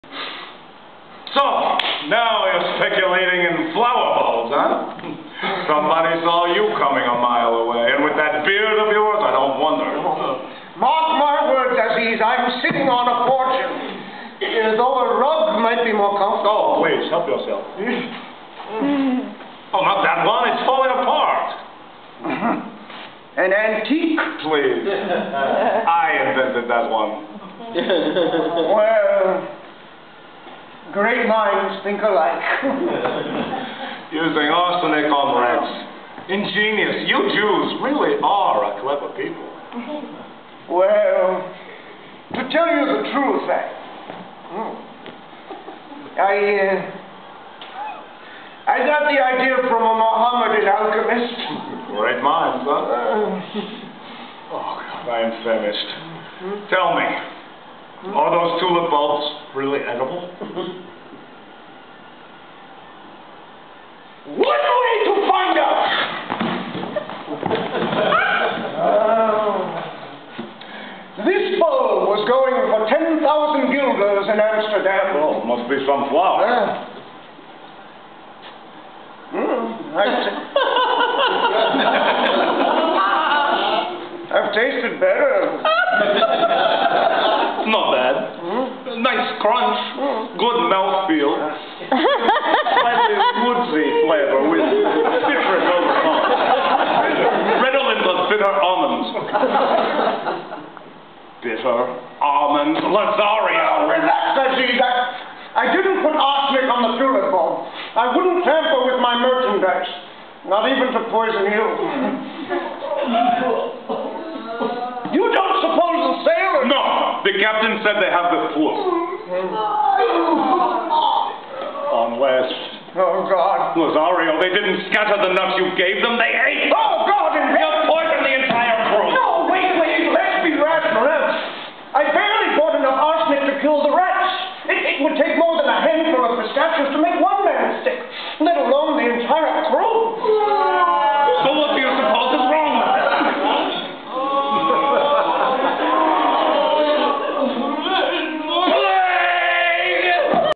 Moises and Ibrahim "hit it off" in another scene from Julia Pearlstein's,"Rat Bastards"
Charles Geyer as Moises Lazzario and Brian Mott as Ibrahim Azziz come to terms with one another in Julia Pearlstein's play "Rat Bastards" directed by Eureka at The Dixon Place Theater NYC 6/05/09. Sets by Philip Pearlstein.